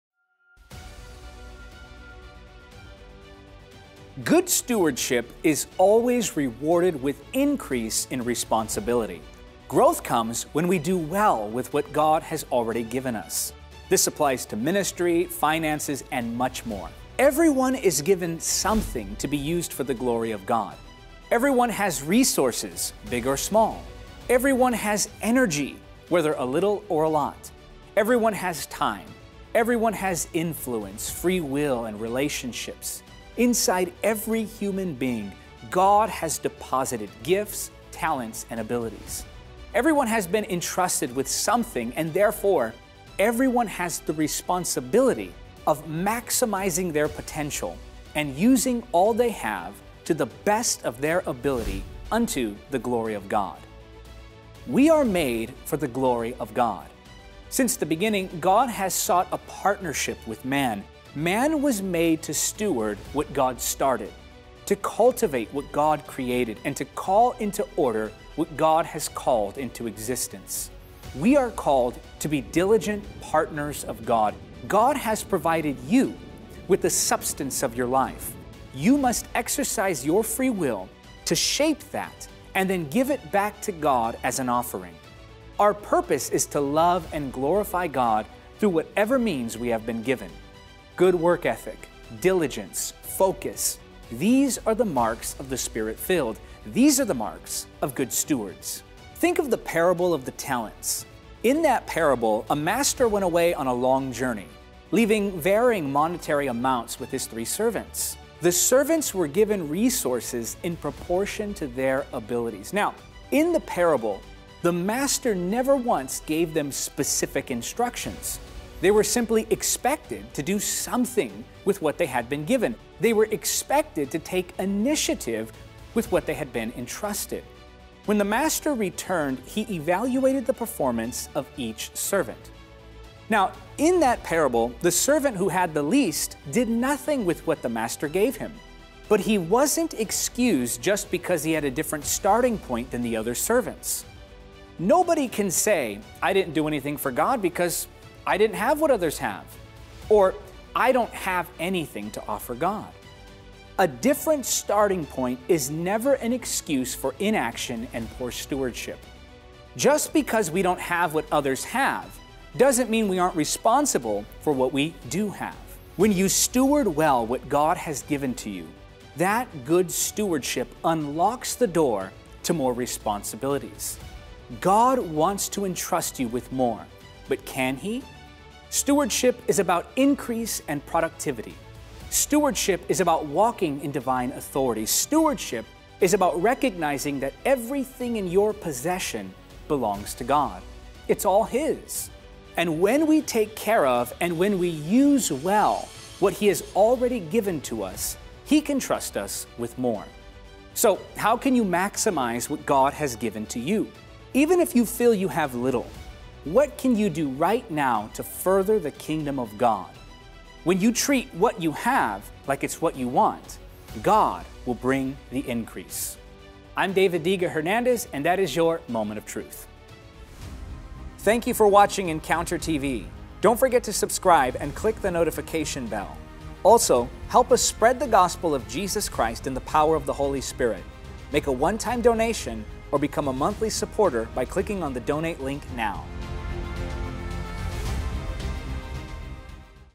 Parable of the Talents: Can God Trust You with More?
God is looking for a partnership in the earth. Can He trust you with more responsibilities? How you use what you have now is a telling sign.